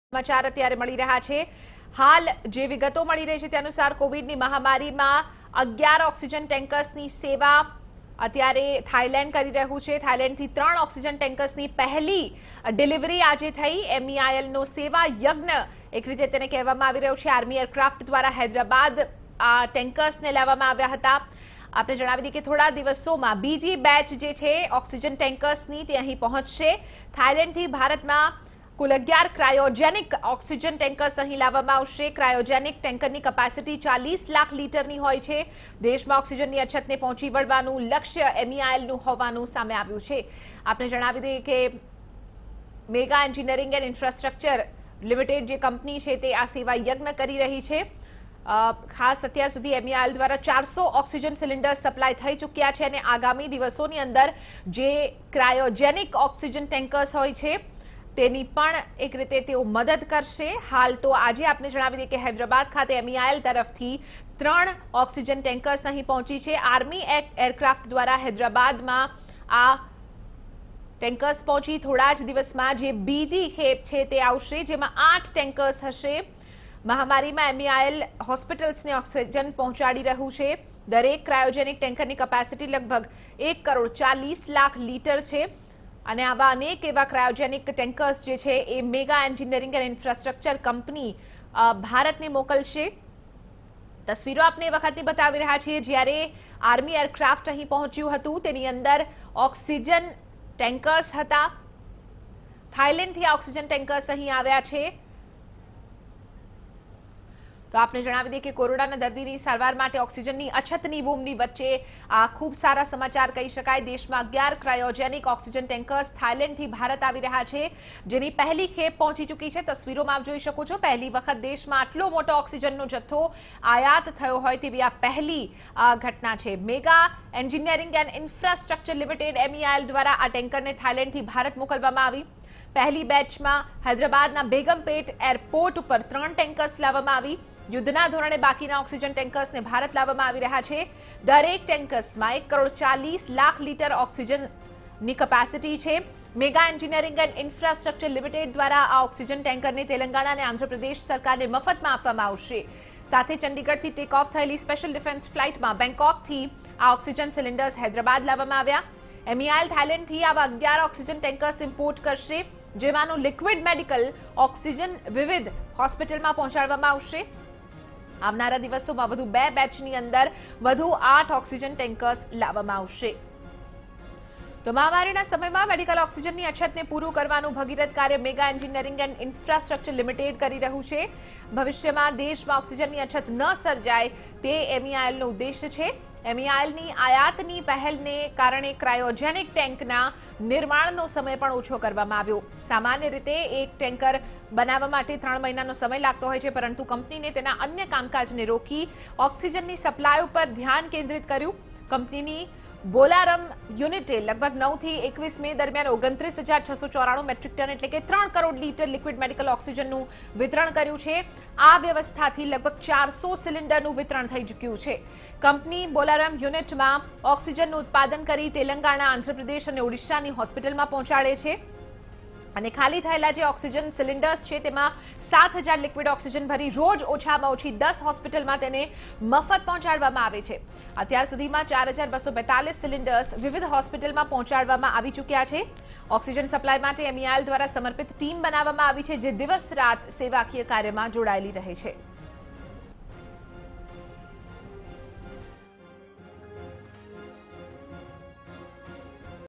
MEIL imports medical oxygen for Telangana free of cost; 3 out of 11 tankers arrive Hyderabad| TV9
Megha Engineering and Infrastructure Limited (MEIL) imports medical oxygen for Telangana free of cost; 3 out of 11 tankers arrive Hyderabad| N05